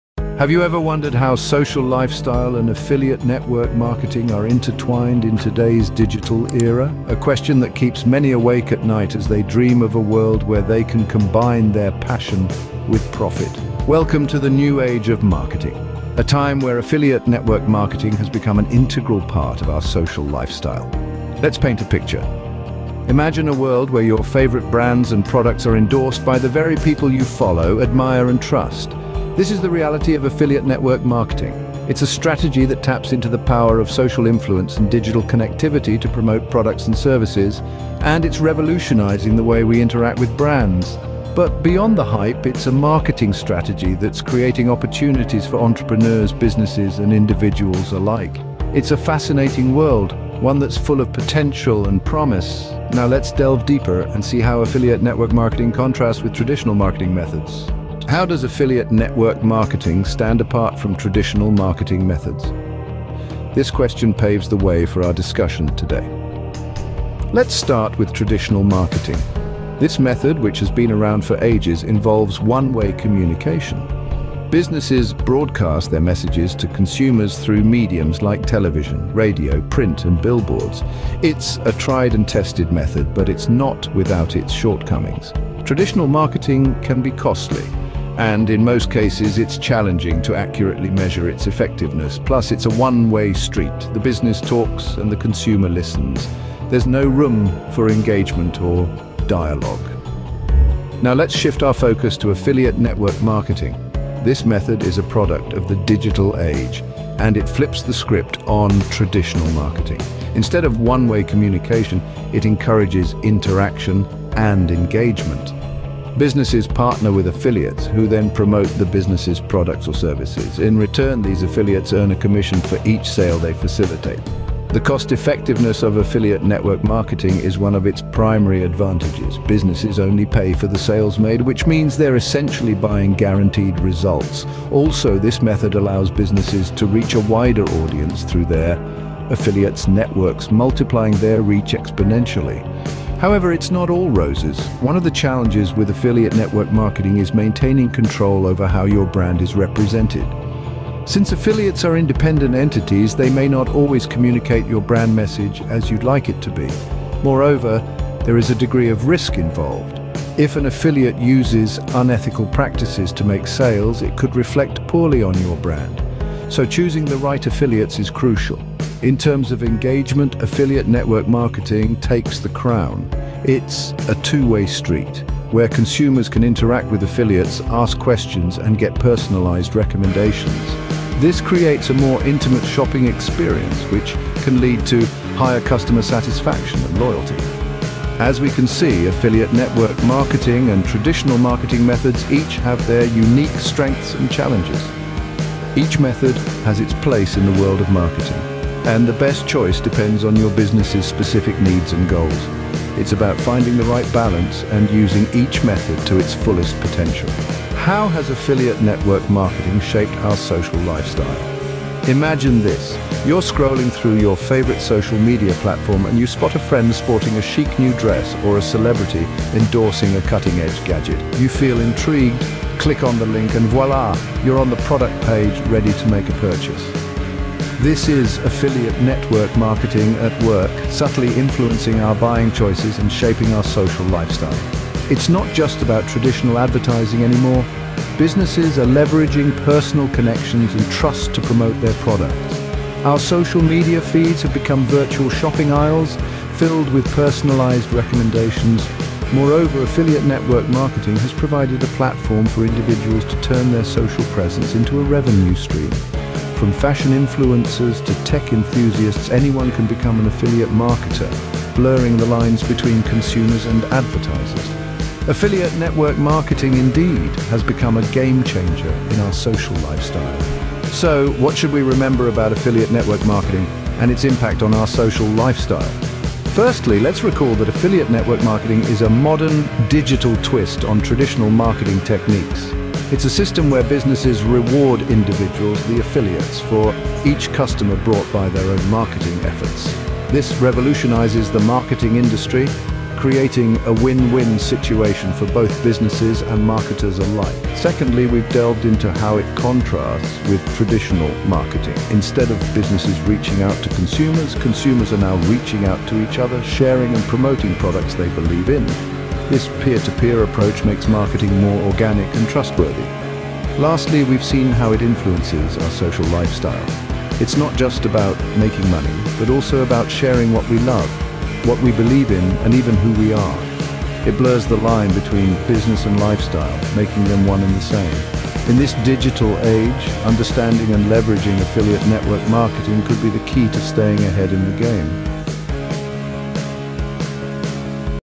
SHORT VIDEO ABOUT @SAO TV AFFILIATE NETWORK MARKETING
In this video you will learn more about @SAO TV  and affiliate network marketing